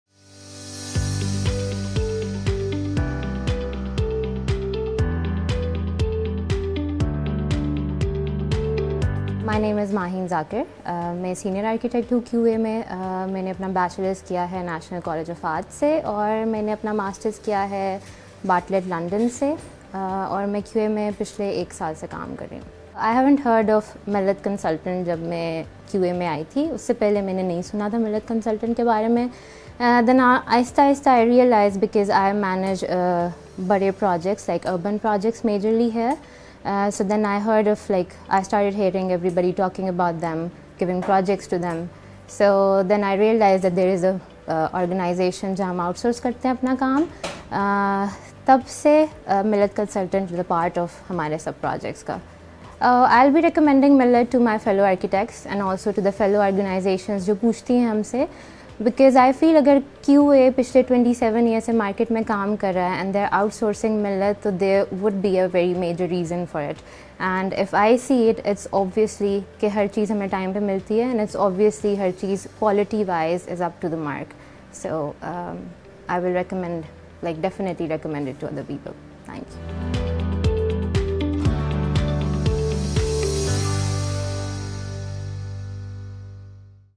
ARCHITECT MAHEEN ZAKIR sharing working experience
Thank you so much of ARCHITECT MAHEEN ZAKIR (Architect – Qadri Associates) for the appreciates of mep mechanical electrical plumbing & firefighting design services and management team of Millat Consultants firm.